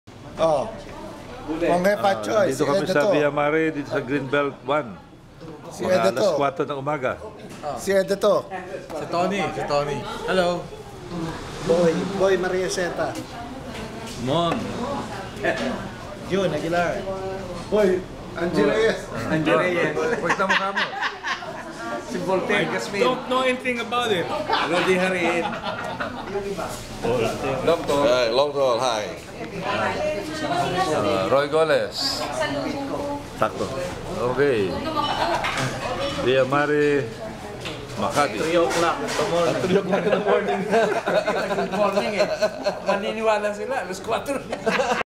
San Sebastian SSC 58-62 get-together, Via Mare, Makati, 26 January 2011
This is the get-together of the members of the San Sebastian College 58-62 namely: Ed Tiotuyco, Tony Chong, Boy Mariazeta, Mon Olmedo, Jun Aguilar, Mon Guevara, Rudy Jarin, Howard Medina and Roilo Golez.. This was held in Via Mare, Greenbelt 1, Makati.